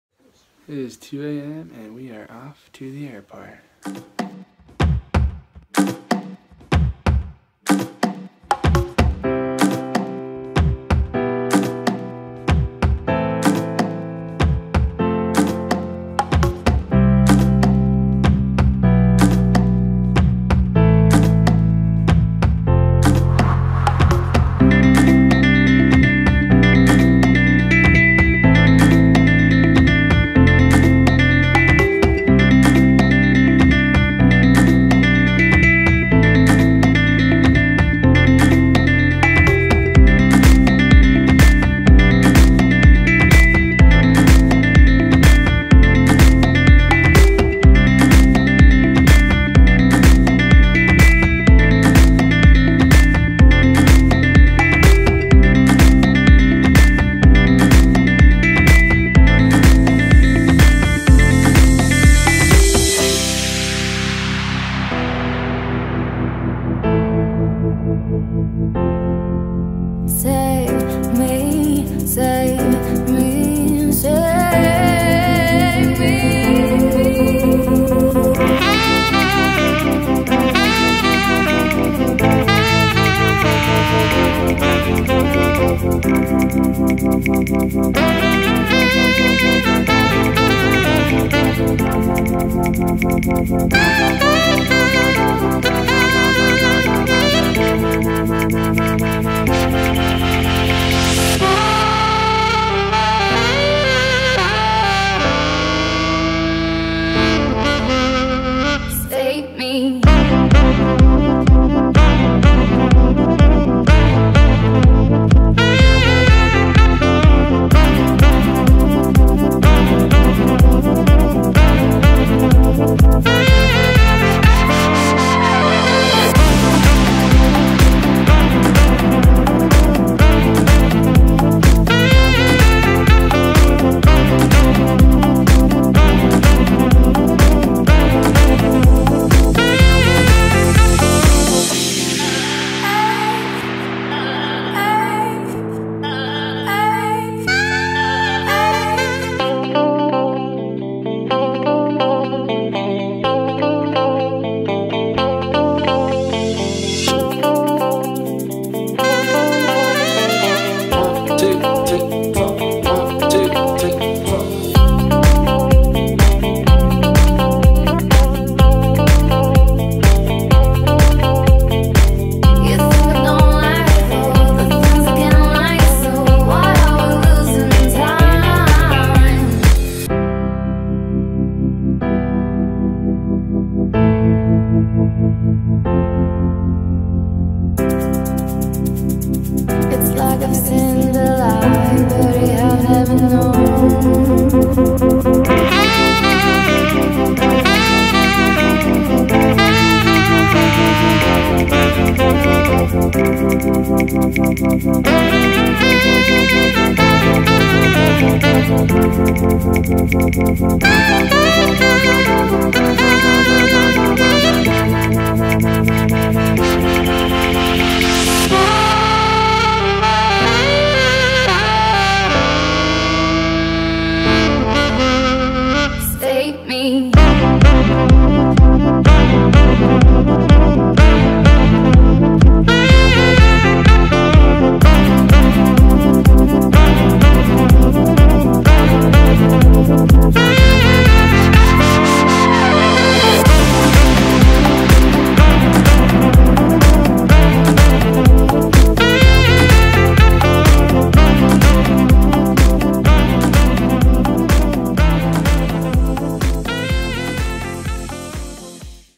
Costa Rica 2017 w/ Mavic Pro and GoPro Karma Grip
Spent 9 days in Costa Rica seeking adventures, new spots, and down time with my new DJI Mavic and GoPro Karma Grip (Hero5). Some of footage could have been better, but I learned from it all. Hopefully my edits will continually improve.